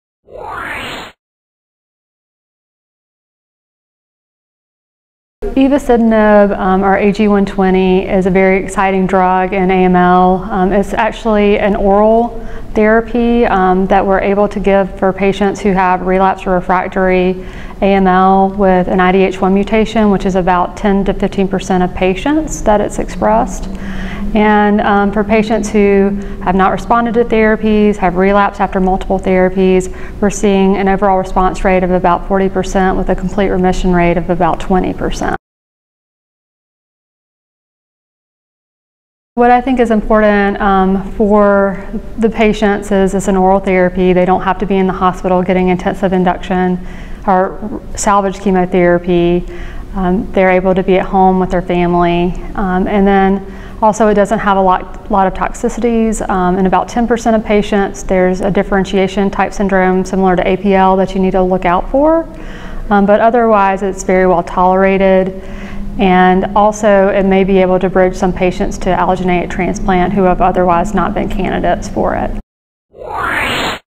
Dr. Mims on FDA Approval of Ivosidenib in Patients With AML
Alice Mims, MD, medical oncologist, Department of Internal Medicine
Division of Hematology, Ohio State University Comprehensive Cancer Center, discusses the FDA approval of the IDH1m inhibitor ivosidenib for the treatment of patients with acute myeloid leukemia (AML).